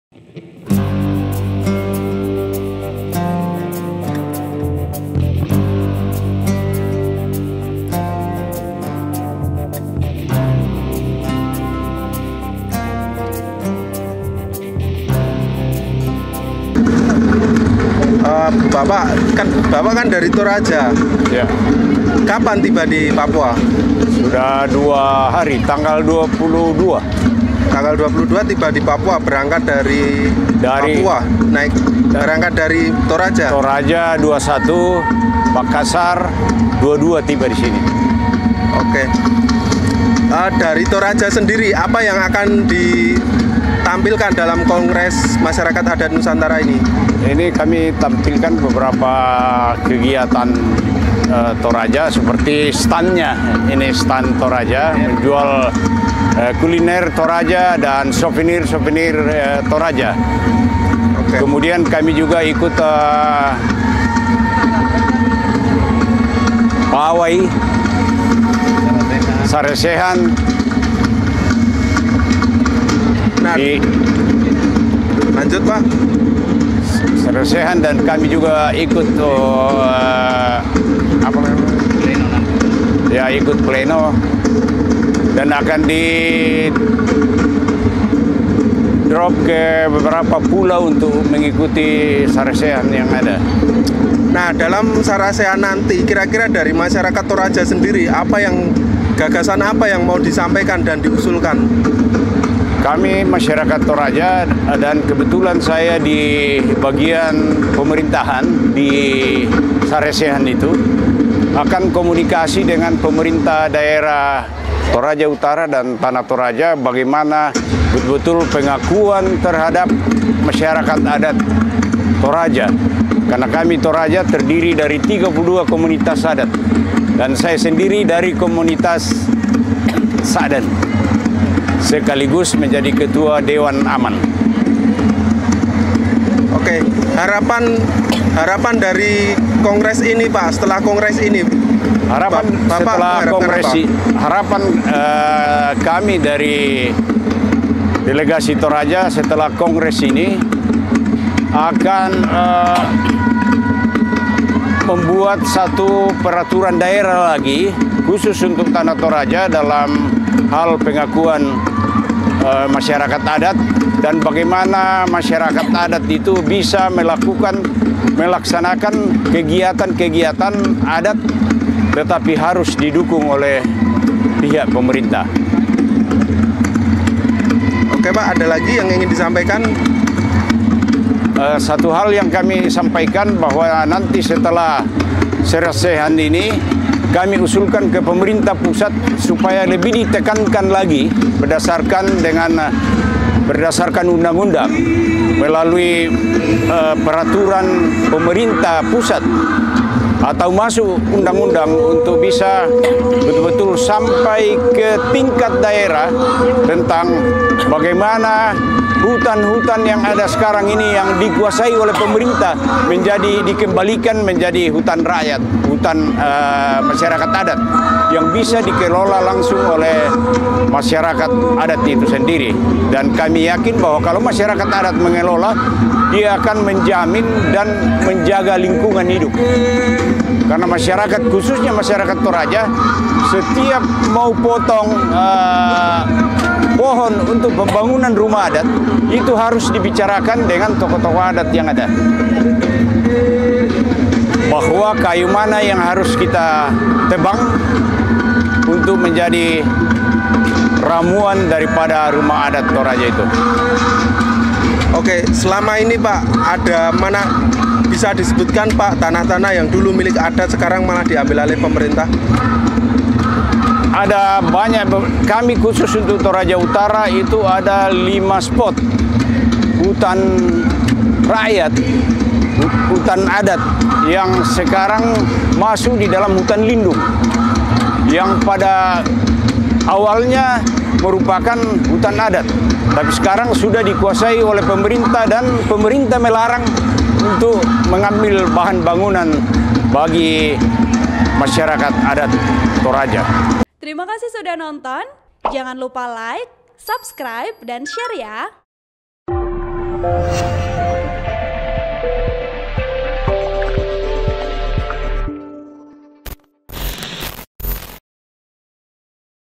Hadir di KMAN VI, Masyarakat Adat Toraja Ingin Hutan yang Dikuasai Pemerintah Dikembalikan ke Adat
Laporan Wartawan Tribun-Video.com/Fikri Febriyanto

TRIBUN-VIDEO.COM- Kongres Masyarakat Adat Nusantara (KMAN) VI digelar di Papua pada 24-30 Oktober 2022.

Kongres tersebut dihadiri perwakilan dari berbagai suku adat di seluruh Indonesia.

Satu di antaranya adalah Suku Toraja dari Sulawesi Selatan.

Dalam kongres ini Suku Toraja meminta pengakuan suku adat mereka terhadap pemerintah.

Pengakuan tersebut tentu saja bukan sekedar tertulis secara administrasi, melainkan mereka meminta kegiatan adat yang mereka lakukan mendapat dukungan dari pemerintah daerah hingga pemerintah pusat.

"Kami masyarakat Toraja dan kebetulan saya di bagian pemerintahan akan komunikasi dengan pemerintahan daerah toraja utara dan tanah utara  mengenai bagaimana pengakuan terhadap masyarakat adat Toraja, karena kami Toraja terdiri dari 32 komunitas adat. Membuat satu perda khusus untuk tanah Toraja dalam hal pengakuan masyarakat adat, dan bagaimana maayarakat adat bisa melaksanakan kegiatan adat tetapi harus didukung pemerintah," terang Lewaran Rantela'bi' - Selaku Ketua Dewan Aliansi Masyarakat Adat Toraya saat Upacara Pembukaan KMAN VI Papua, Senin (22/10/2022).

"Setelah sarasehan ini kami usulkan pemerintah pusat supaya lebih ditekankan lagi berdasarkan UU, bagaimana hutan2 yang dikuasai pemerintah bisa dikembalikan lagi menjadi hutan rakyat, dan kami yakin kalau masyarakat adat yang mengelola akan menjamin dan menjaga lingkungan hidup," terang Lewaran.

Lewaran menjelaskan, saat dirawat masyarakat adat, ketika hendak menebang pohon untuk keperluan, maka akan ada musyawarah dengan Kepala Adat.

Sehingga terkait perusakan hutan yang merugikan alam bisa terhindar.

Sebagai contoh, selama ini di Toraja Utara ada 5 spot hutan rakyat yang sudah masuk di dalam hutan lindung, padahal sebelumnya hutan tersebut merupakan hutan adat.

Masyarakat adat yang ingin mengambil bahan dari hutan pun kesulitan ketika mereka hendak membangun rumah adat mereka.

TRIBUN-VIDEO.COM/FIKRI FEBRIYANTO